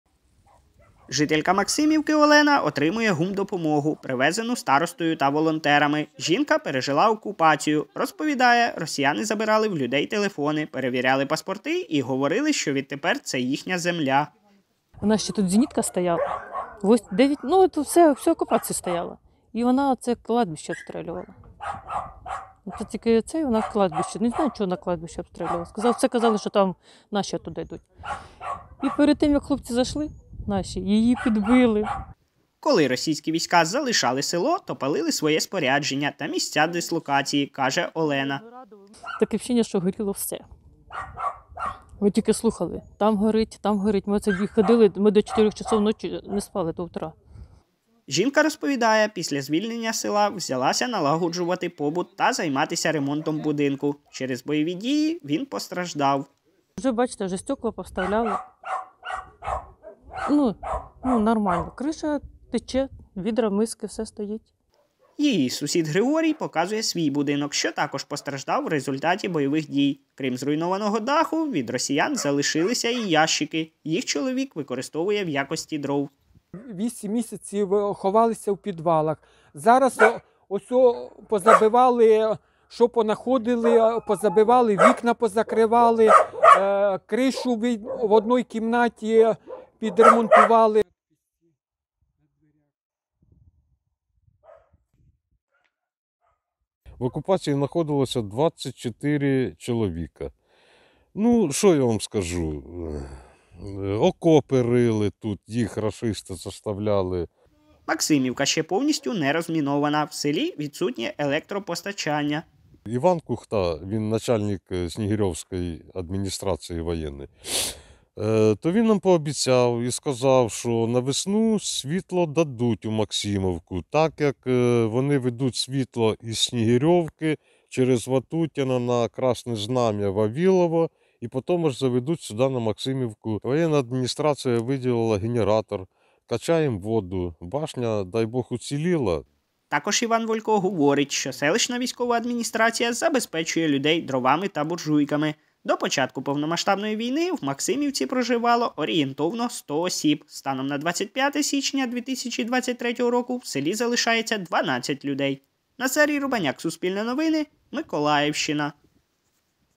Жителі села на Миколаївщині розповіли про пережите в російській окупації
Село Максимівка Первомайської селищної громади, що на Миколаївщині, перебувало в російській окупації з 9 березня до 10 листопада 2022 року. До початку повномасштабної війни в Максимівці проживало орієнтовно 100 людей, станом на 25 січня 2023 року в селі залишається 12 жителів.